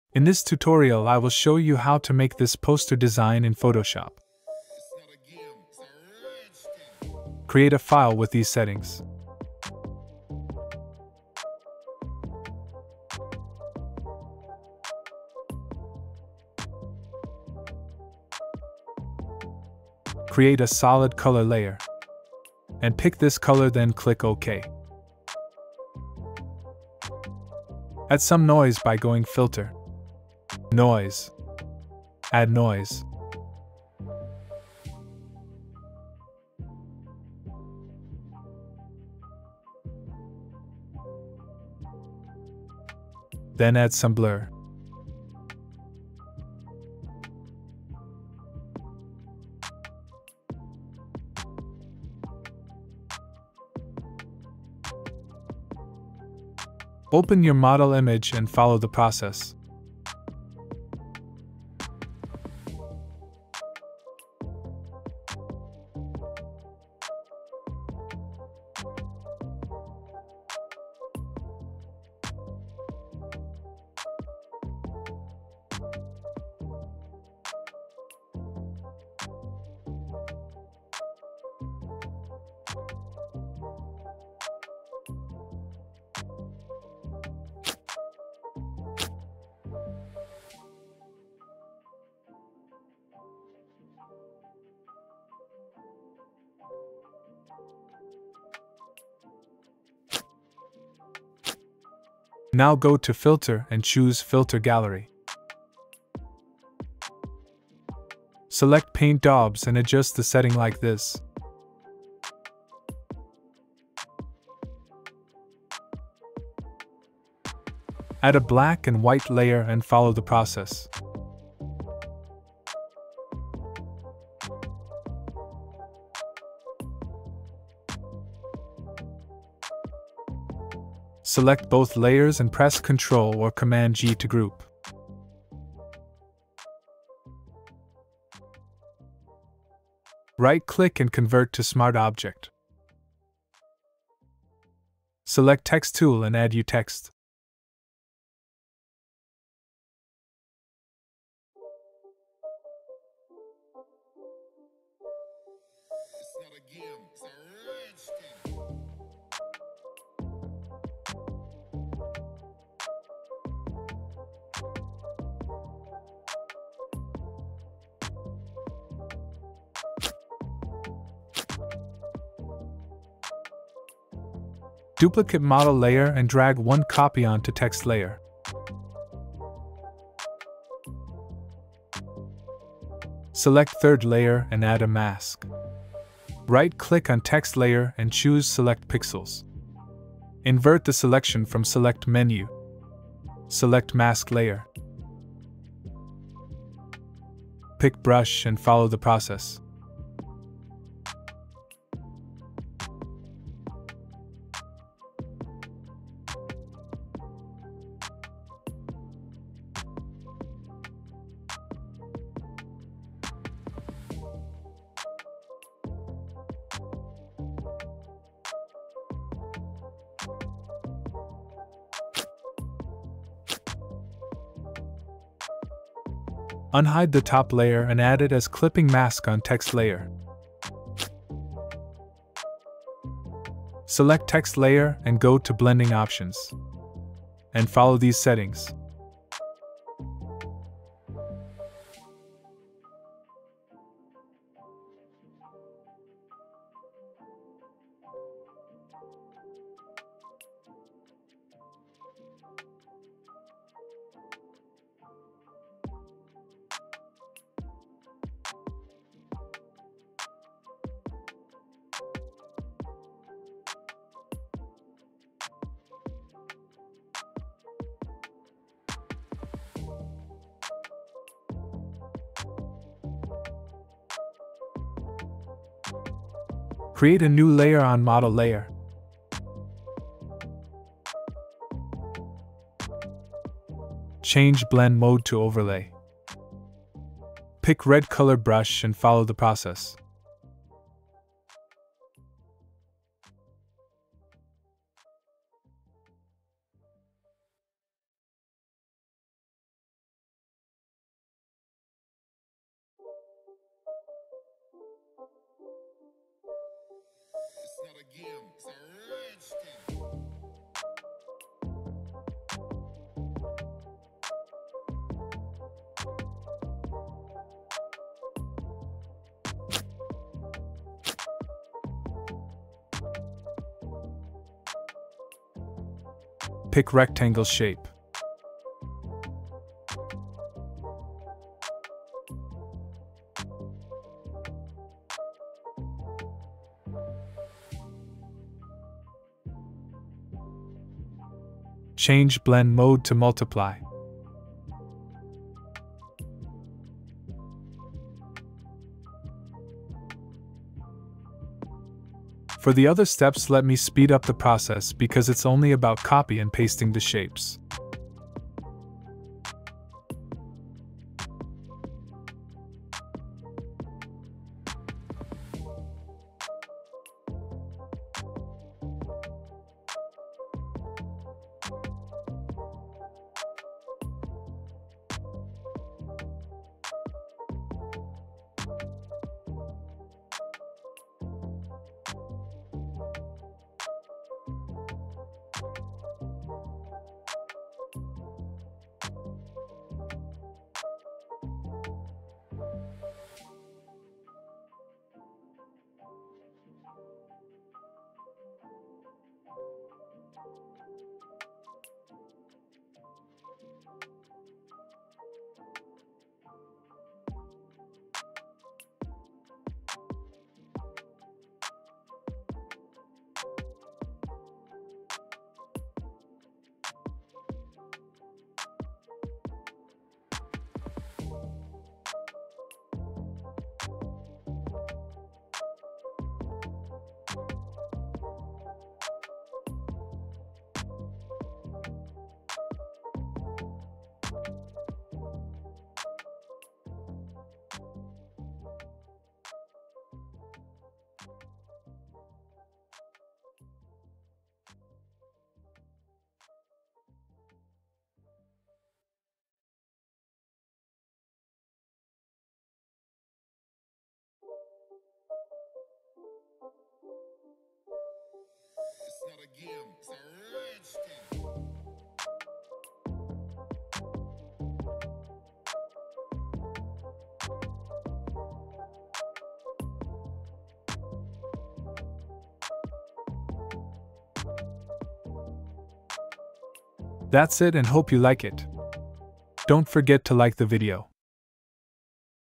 Tutorial for beginners | Poster Design Tutorial in Photoshop
How to make top modeling poster design using adobe Photoshop.
I Used Photoshop 2023 version.